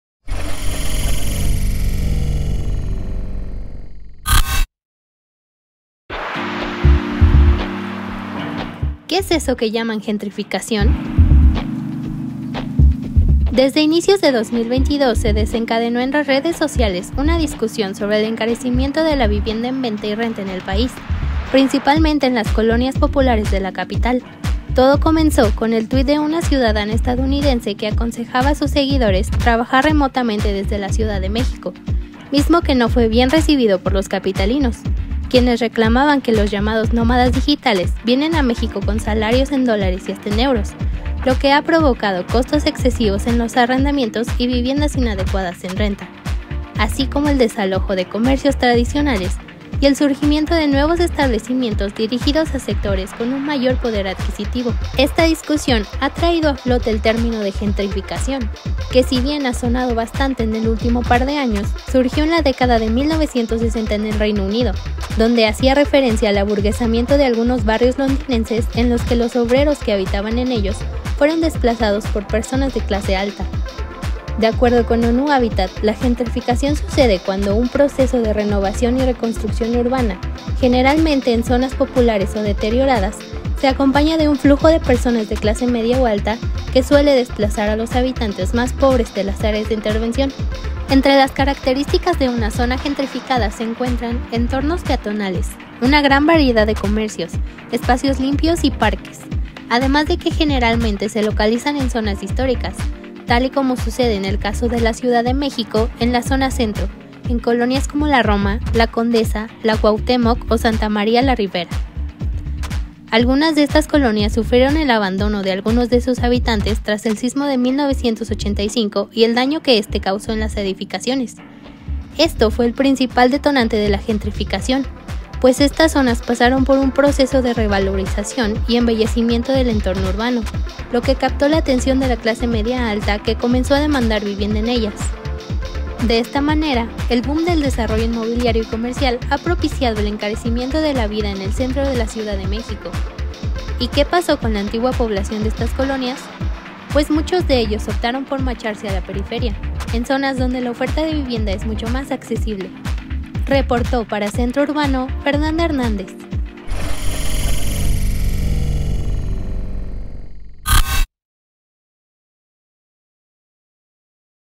¿Qué es la Gentrificación?
¿Qué es la Gentrificación? ¿Qué pasa con la Gentrificación en México? ¿Cómo se involucran nómadas digitales y esquemas como Airbnb en este fenómeno?

El tema de la Gentrificación ha estado presente en la agenda mediática en lo que va de 2023. Pero, ¿qué es y cómo ha evolucionado este fenómeno? Aquí te contamos